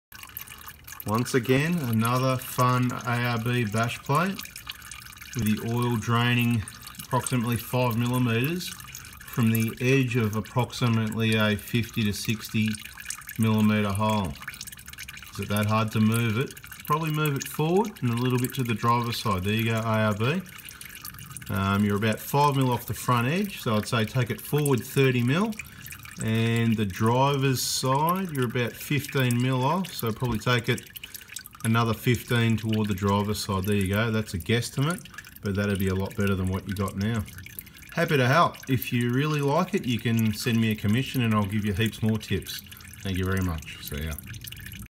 Arb bash plates weak 3mm
Can’t even get the hole is the right spot.
Cumon! Usually easily bent & hard to remove & refit.
Stick with kaon.com.au for under body protection